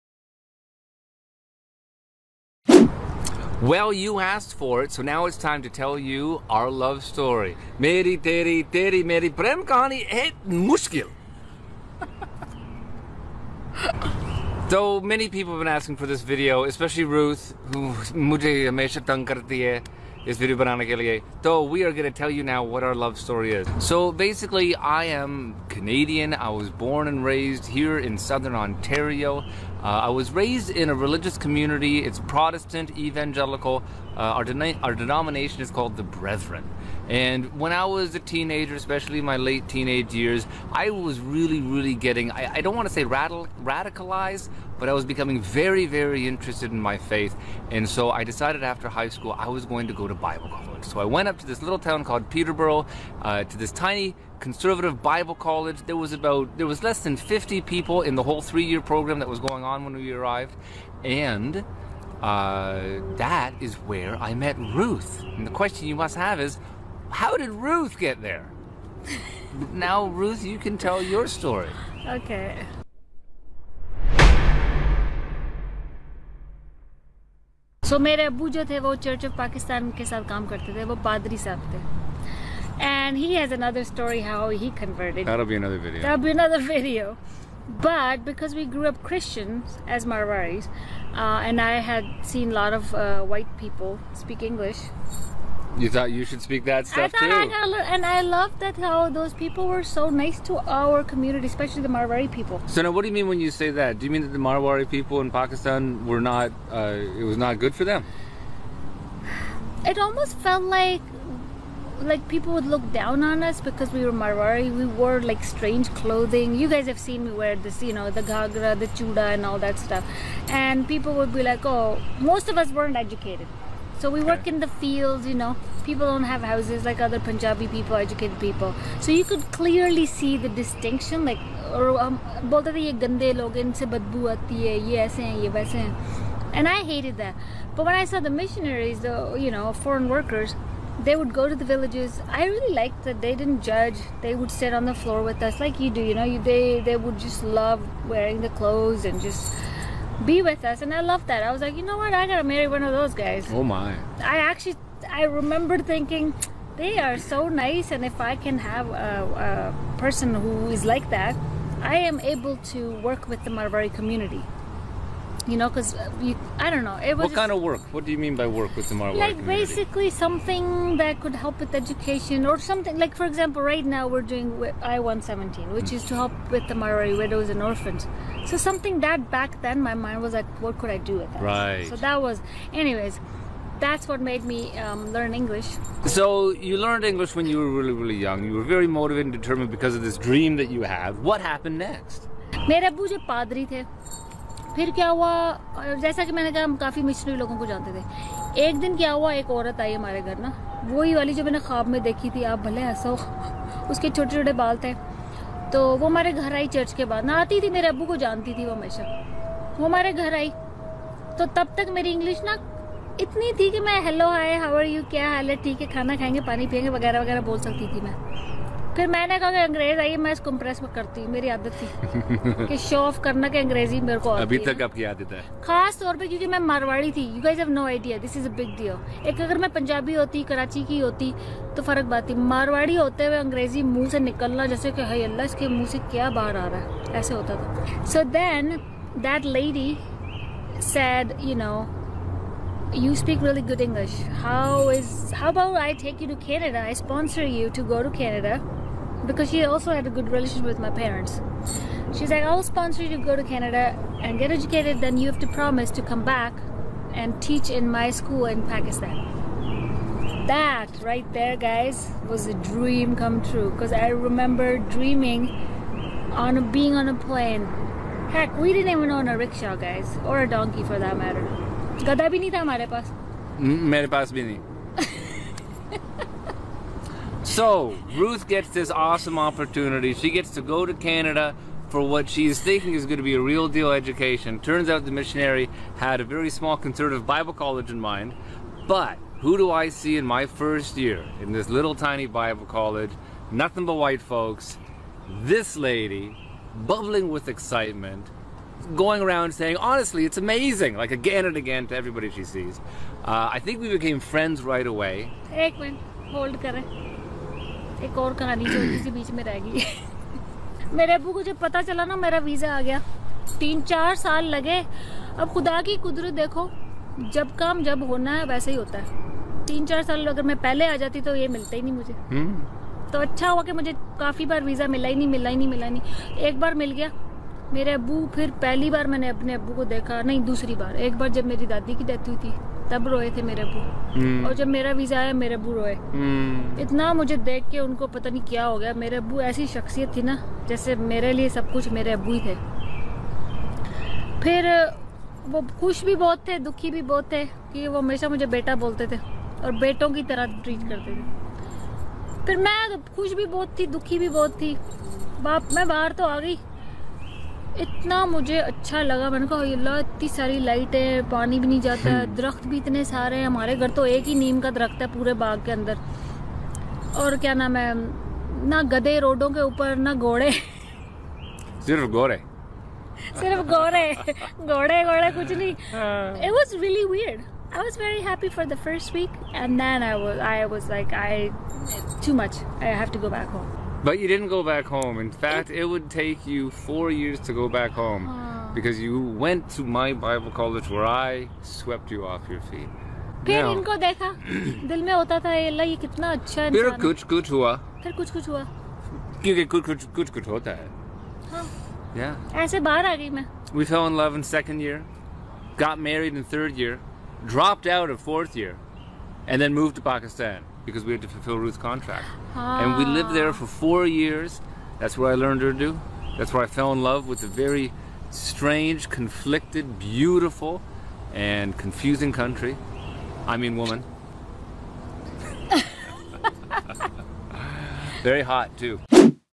Ruth and Matt's Love Story--Part One
The seven-minute version of how Ruth and Matt met way back in the first years of the century!